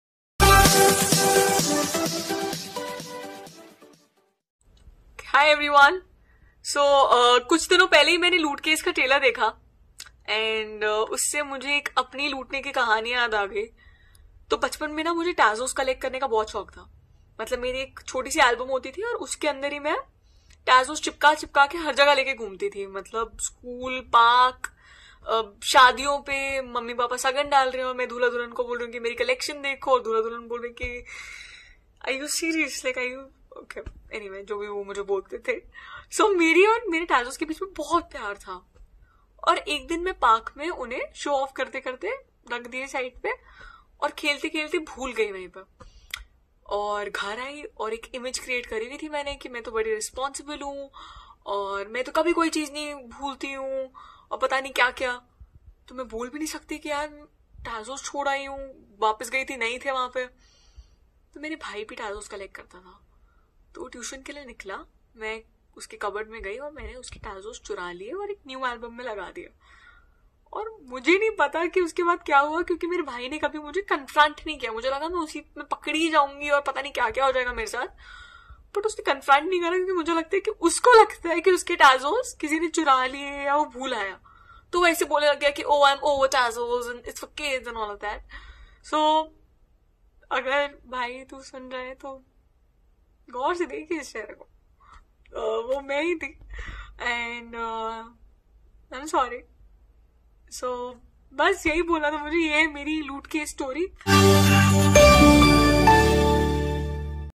Radhika Madan shares about her looting incident
Watch young Indian Actress Radhika Madan talks to her fans about her stealing incident.

Dont forget to press the notification button as well.